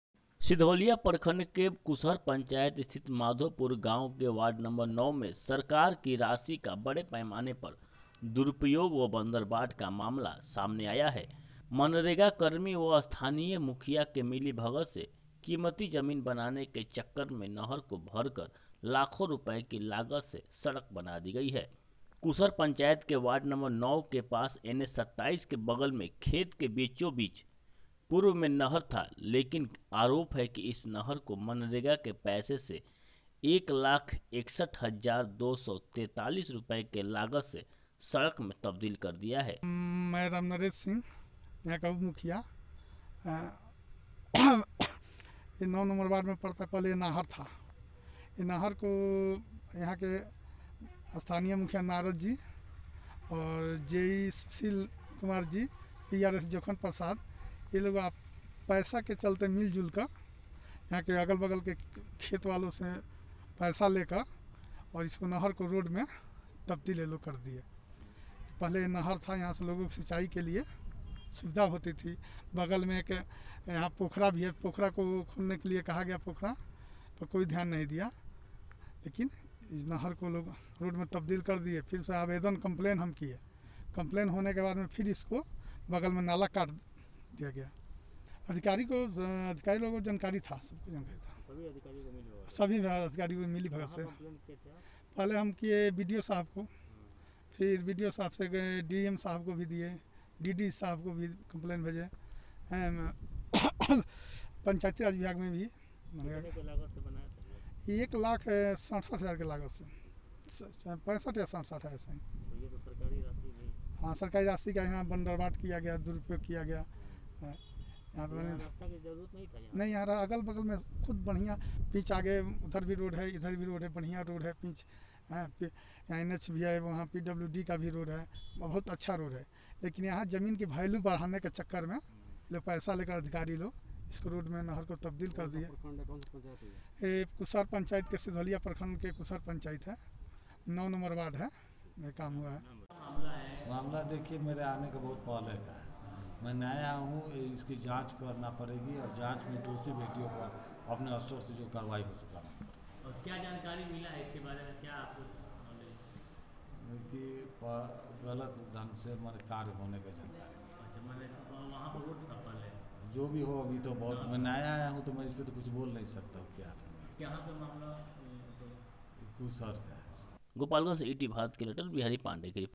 Scam by mukhiya and mgnrega workers in Gopalganj
जिले में मनरेगा योजना में लूट का मामला सामने आया है. ताजा मामला माधोपुर गांव का है. जहां कीमती जमीन बनाने के लिए मनरेगा कर्मी और मुखिया के मिली भगत से नहर पर सड़क बना दी गई.